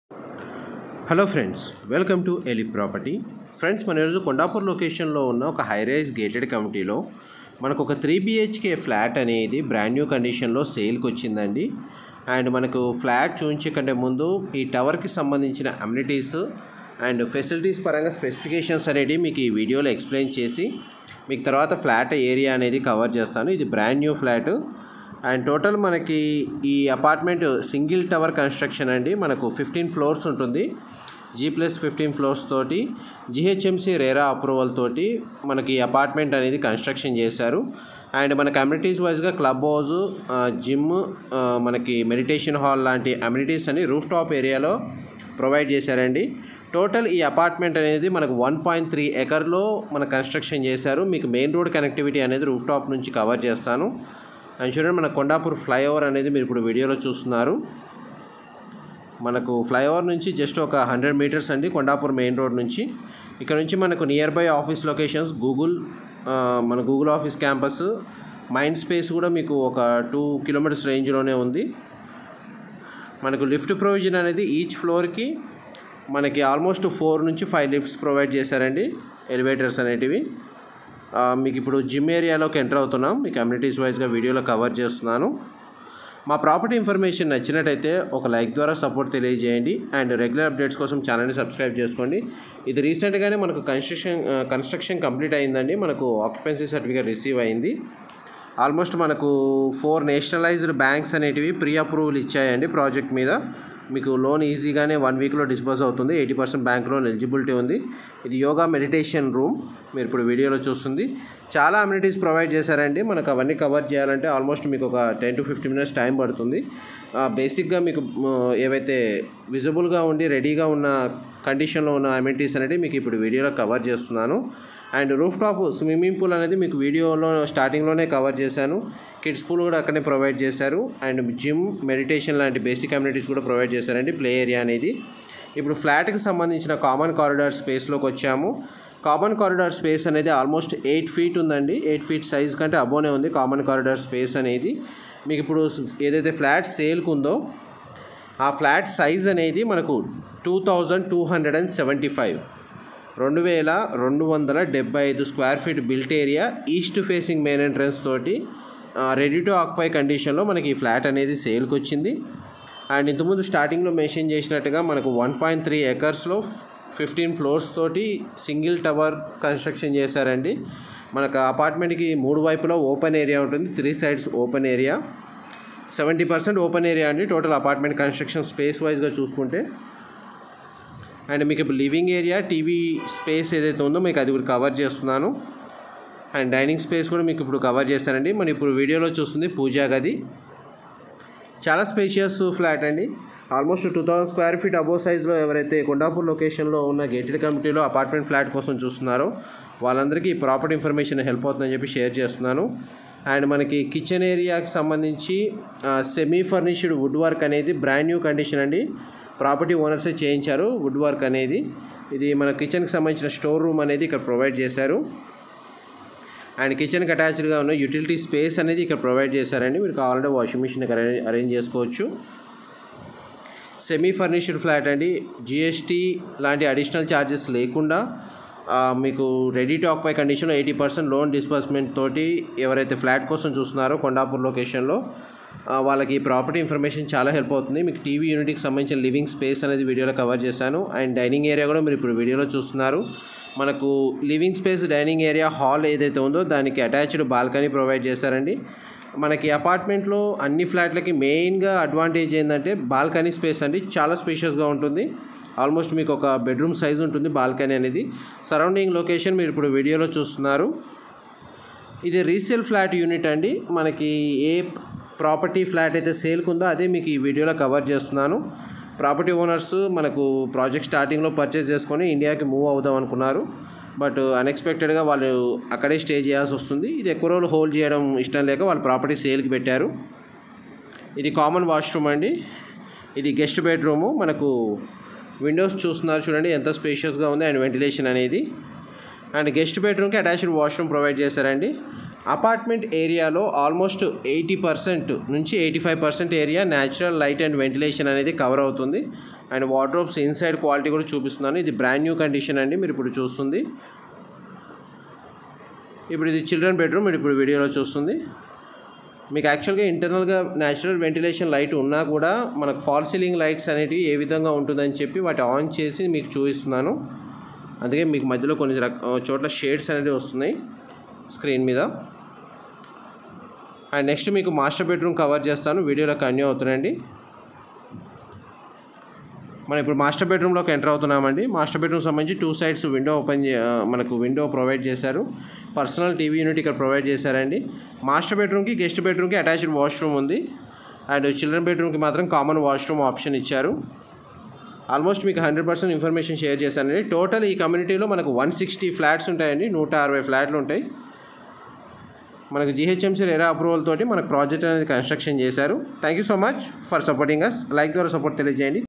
2275 SQ FT || FLAT FOR SALE || HYDERABAD || ELIP PROPERTY
2275 SQ FT FLAT FOR SALE IN GATED COMMUNITY FLAT FOR SALE HYDERABAD ELIP PROPERTY

GHMC APPROVED 15 FLOOR APARTMENT

2 CAR PARKING AVAILABLE

70 % OPEN AREA IN COMMUNITY

PRICE : 8 K INR PER SQ FT ( ALL INCLUDED )